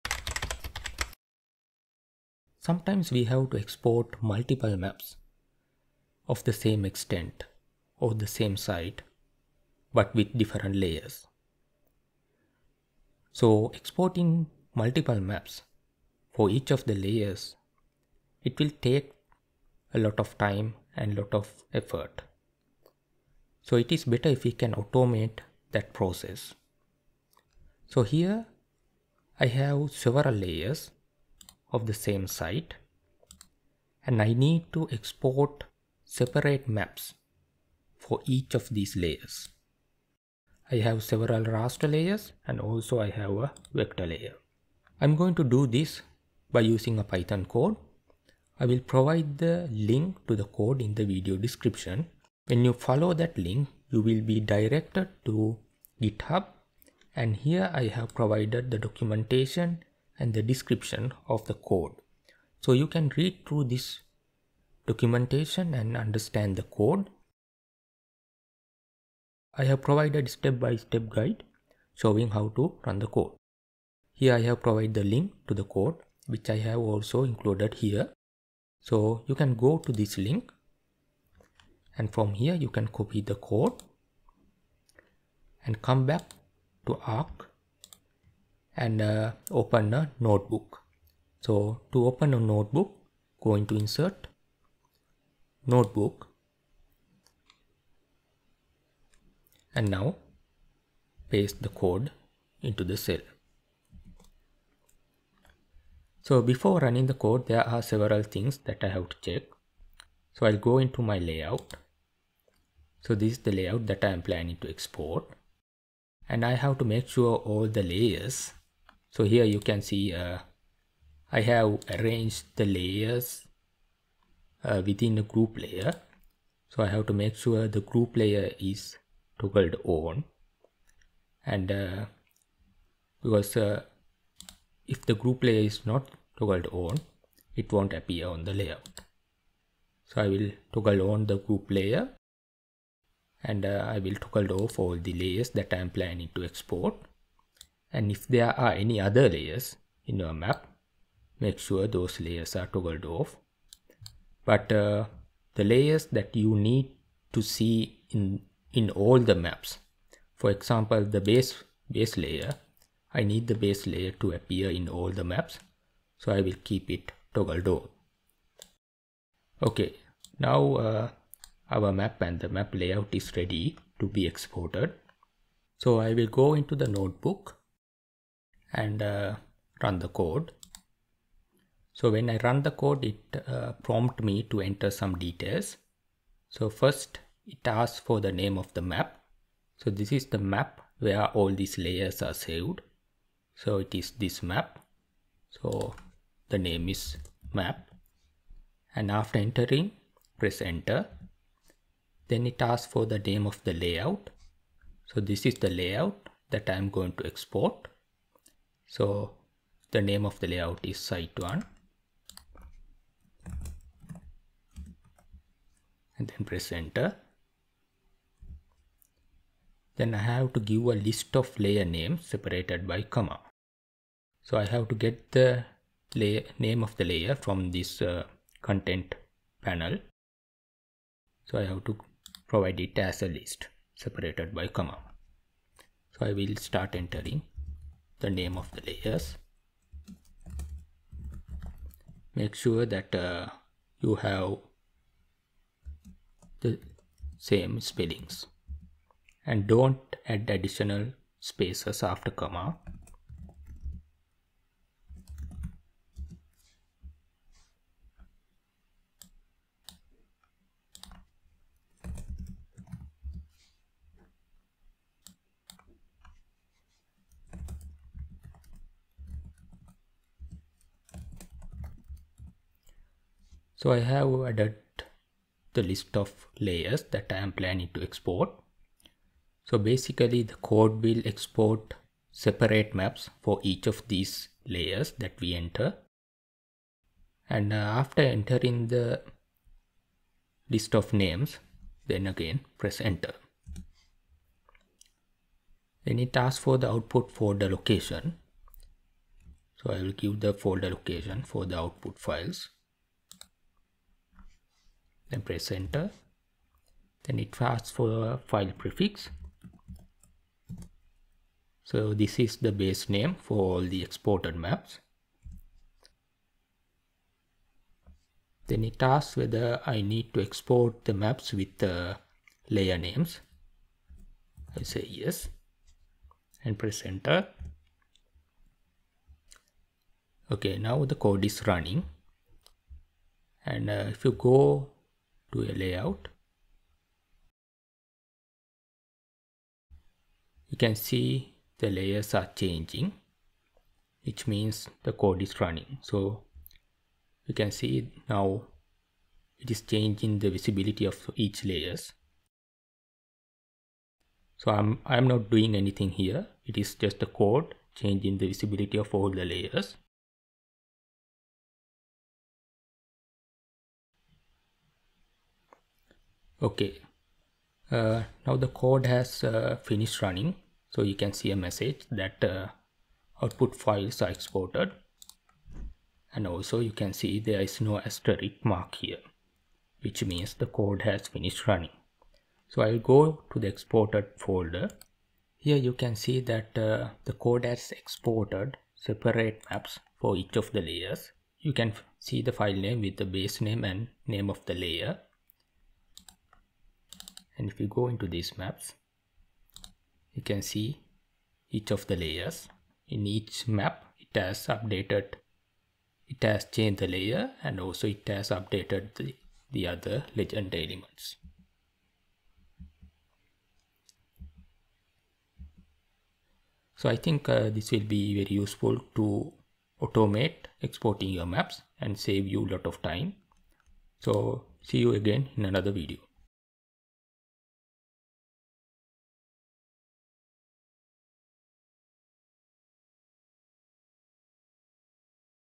ArcGISPro: Thematic map series using a python code
This video will show you how to automate exporting maps in ArcGISPro. Here a Python code is used to loop through a list of layers, toggle their visibility, and export the map layout to a PNG file for each layer.

Here is the link to the python code (in GitHub)

If you want to jump to a particular chapter you want.
0:00 - Intro & precheck
3:00 - Code Execution
6:00 - Output

Feel free to add your comments and suggestions in the comment section.

Here is the link to a full playlist of MS Word tutorials that will be useful for your reports and thesis.